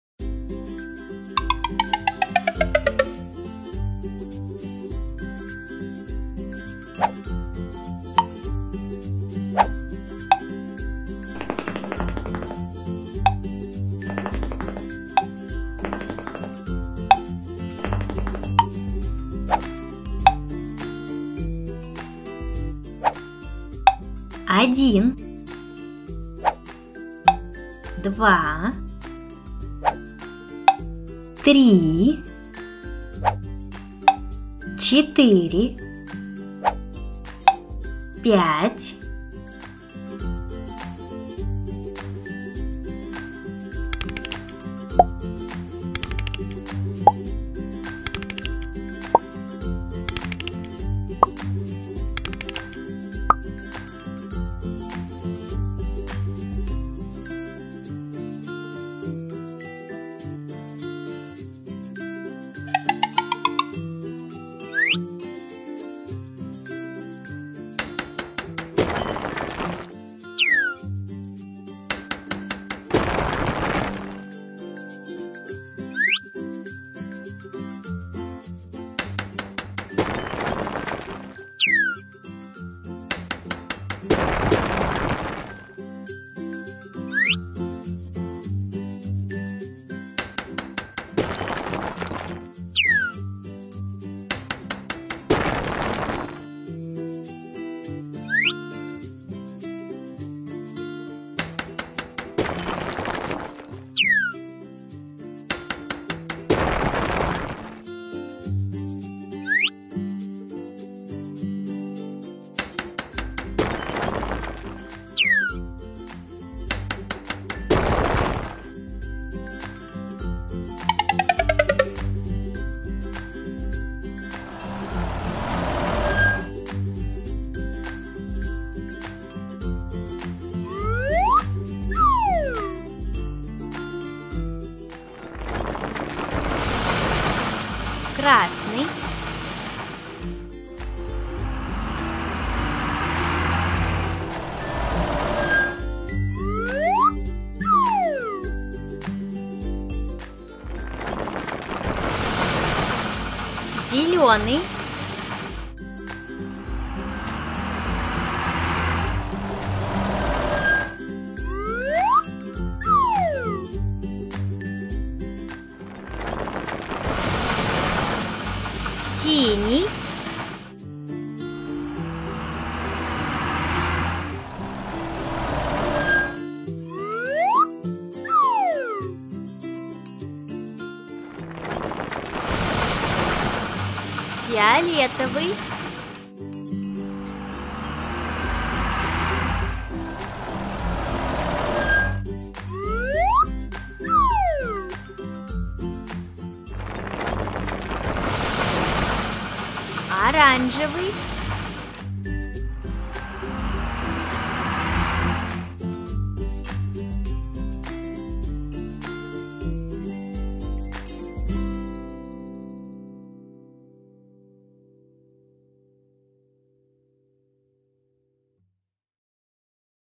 Яйца с сюрпризом и грузовички Учим цвета и цифры Развивающий мультик для детей.
В этом развивающем мультфильме яйца с сюрпризом и грузовички помогут ребятам выучить цвета и цифры от 1 до 5.